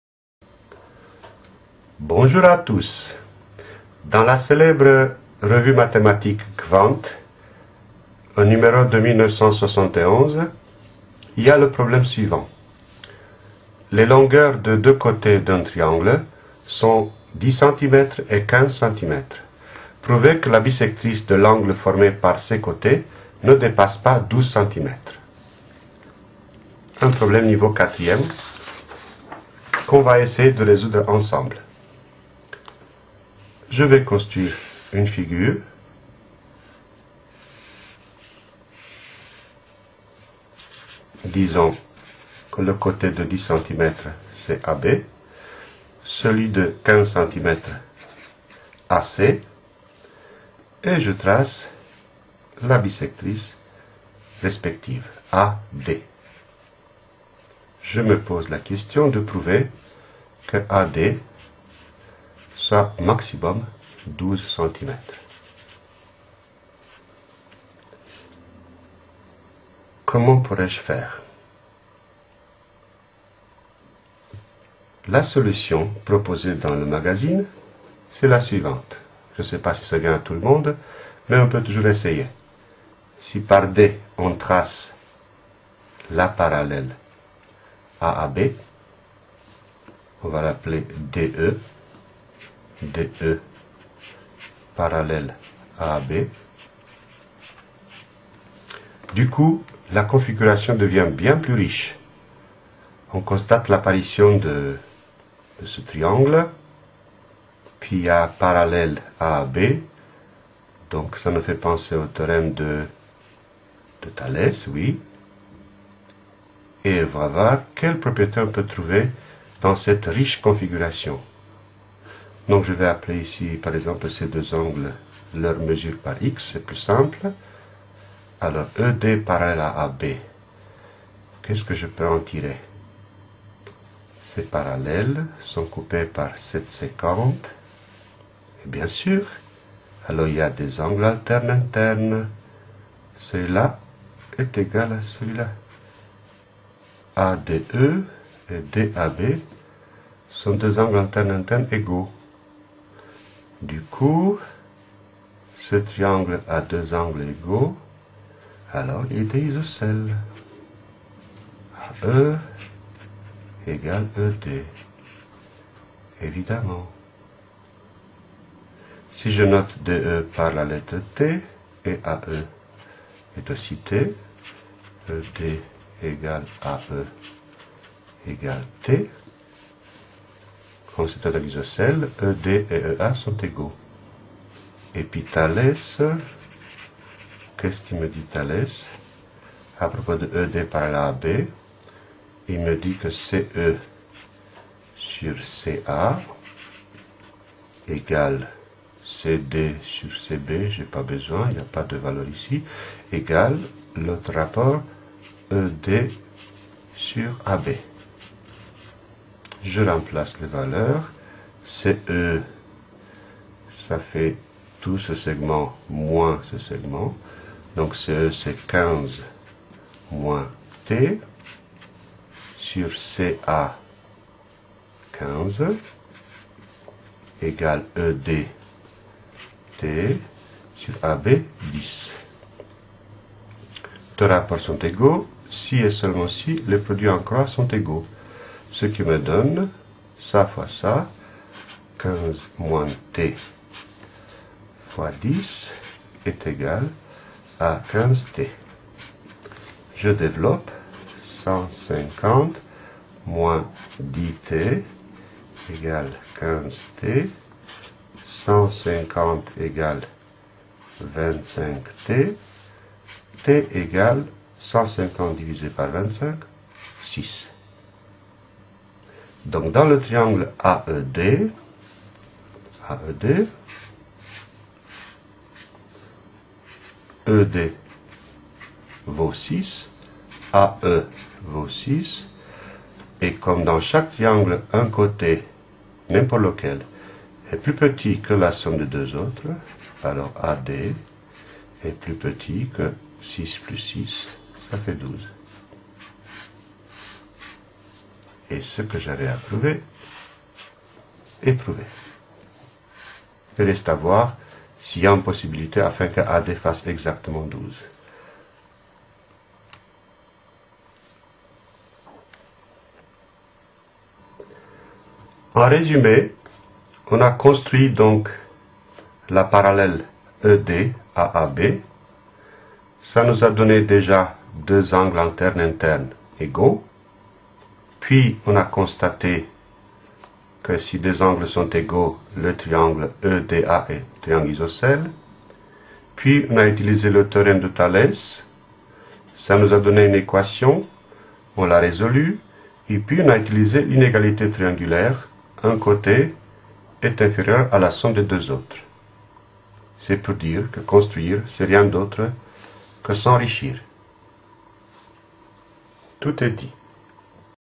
36. 4e- Une bissectrice limitée
Construire une parallèle et tout devient plus riche !
Il s'agit d'un petit bijou mathématique résolu pour les élèves
à partir de la 4ème.